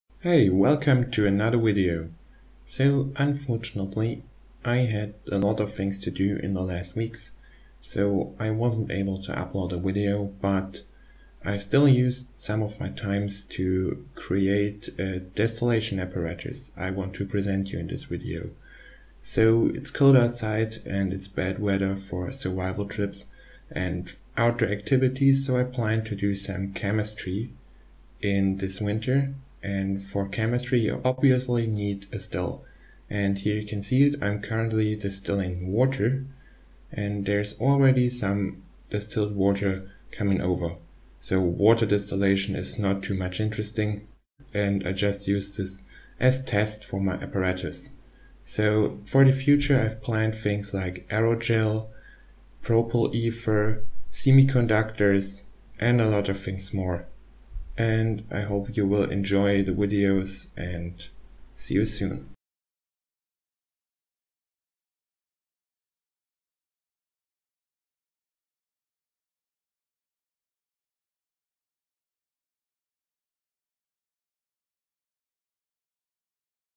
DIY Destillation Apparatus (And Future Projections)
See my homemade still. Additionally I give a quick overview over the future of Shrom Lab.